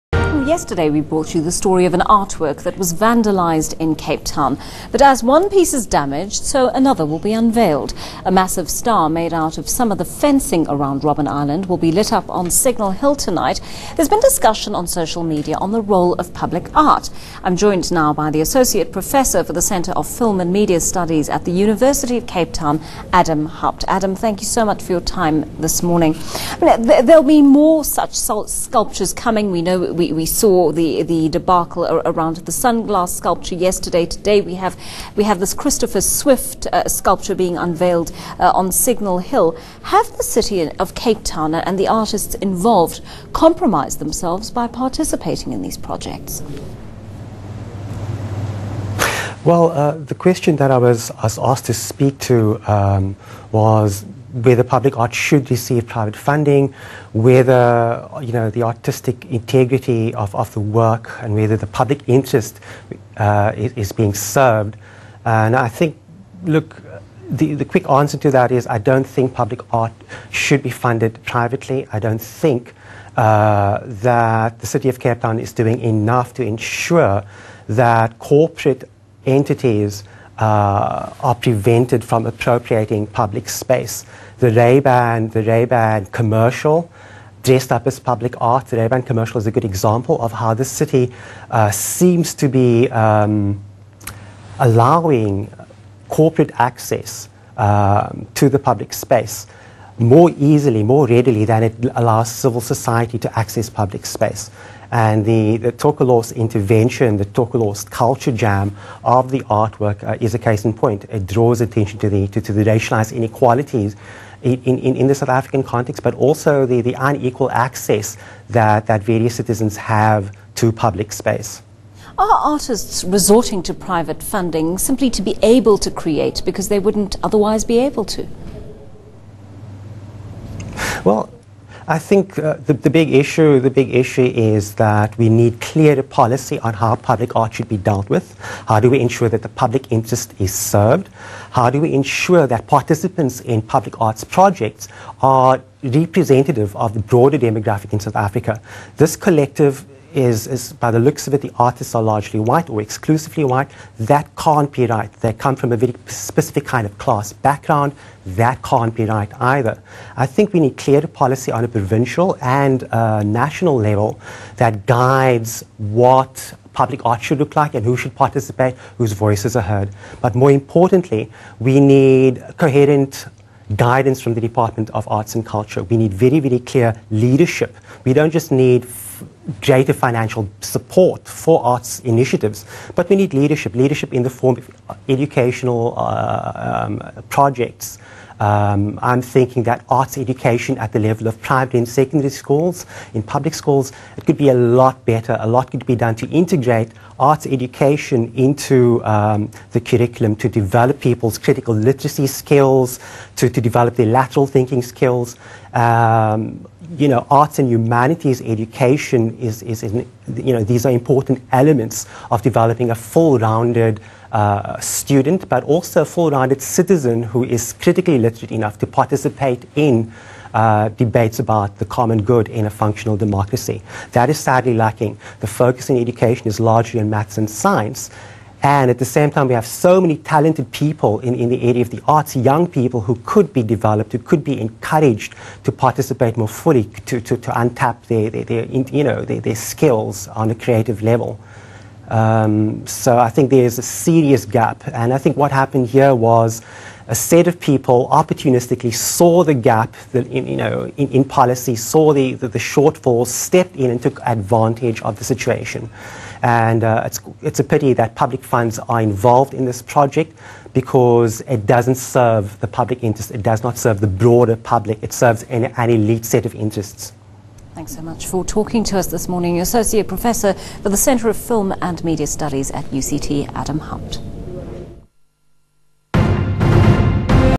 Tokolos Stencils vs Perceving Freedom on eNCA
an eNCA interview with Adam Haupt on the Tokolos Stencils culture jam of Michael Elion's Perceiving Freedom, a public art installation on the Sea Point Promenade in Cape Town NCA RayBan (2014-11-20).